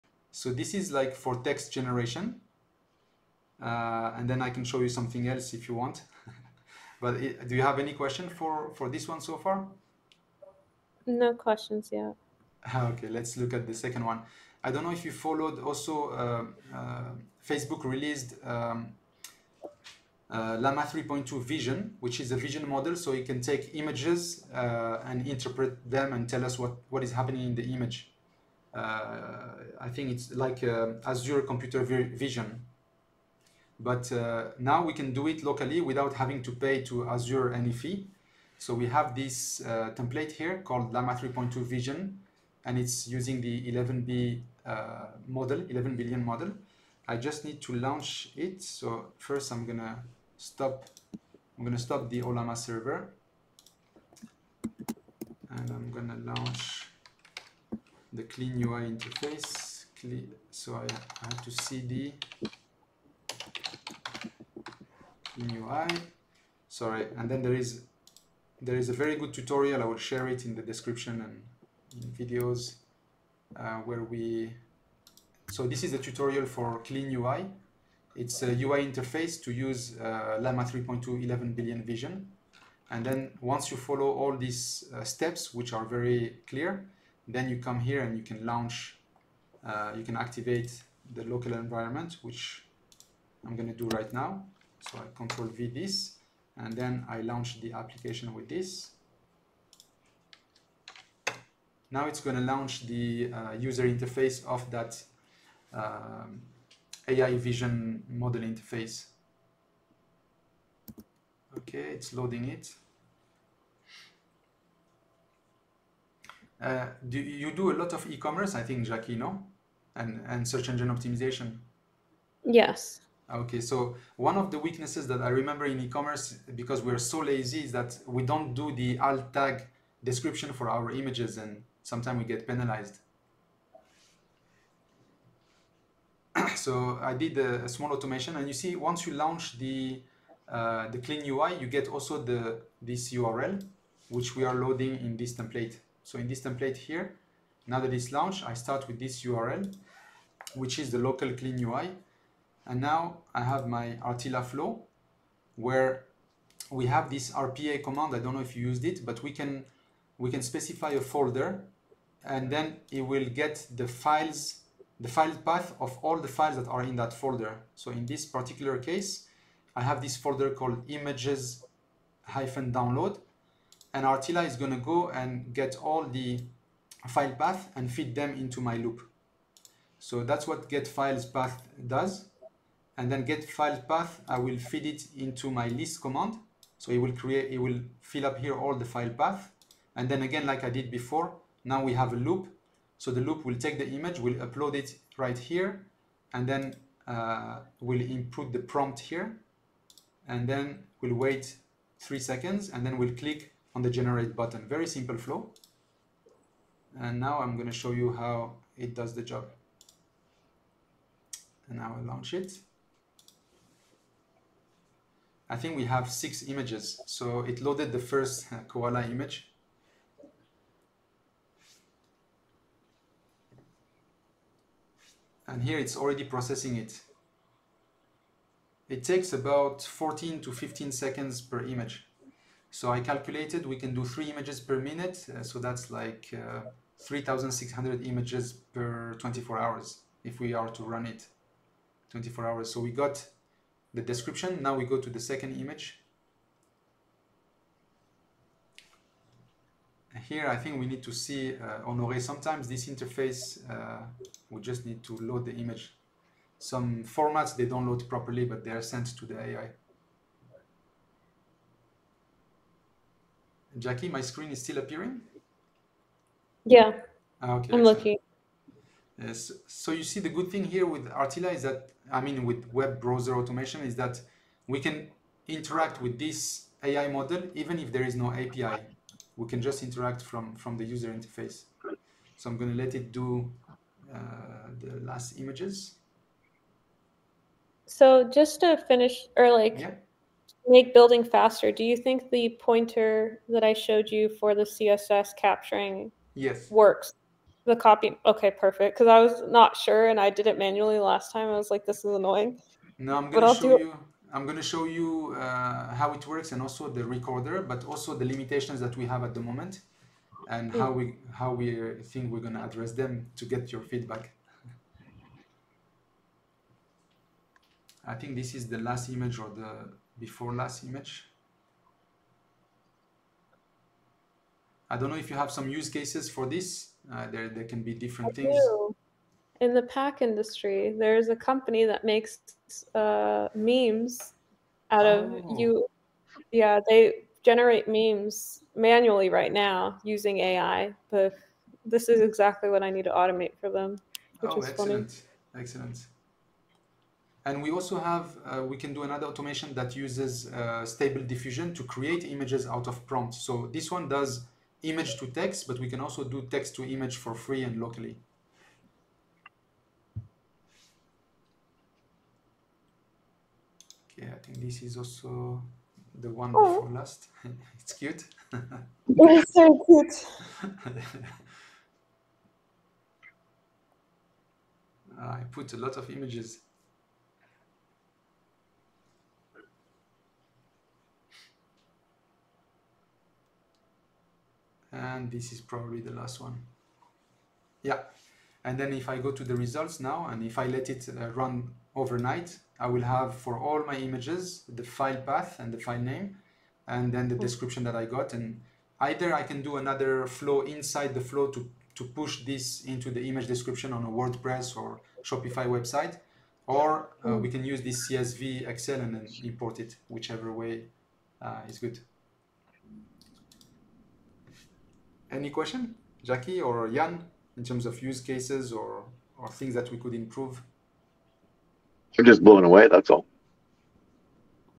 aiautomation for Llama 3.2 Vision to create Unlimited Free Image Descriptions
Automate the use of Image to text Ai Generation using Llama 3.2 Vision latest LLM from meta right on your computer using an RTX 3090. With RTILA we are able to automatically pull all the images of a given folder and upload them to clean-Ui local LLM interface for Llama 3.2 vision 11b model to get nice Alt Tag SEO optimized images descriptions for your eCommerce store such as WordPress or Shopify.

2) After installing make sure you cd into the folder using this command line: cd clean-ui
3) Activate the virtual environment using this command line: .\venv-ui\Scripts\activate
4) Launch the interface using this command line: python clean-ui.py
5) Only start this template when the interface is ready via this URL